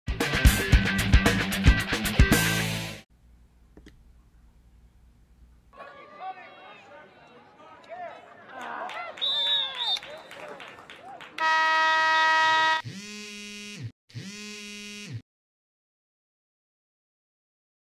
CMSportsNet Clipzone: Gerstell continues their strong lead over FSK as Carter Eberle scores
Check out this Clipzone play in the Gerstell vs Francis Scott Key Girls Lacrosse Game on 3/22/2024. If you liked that clip, why not follow our LIVE coverage of the game?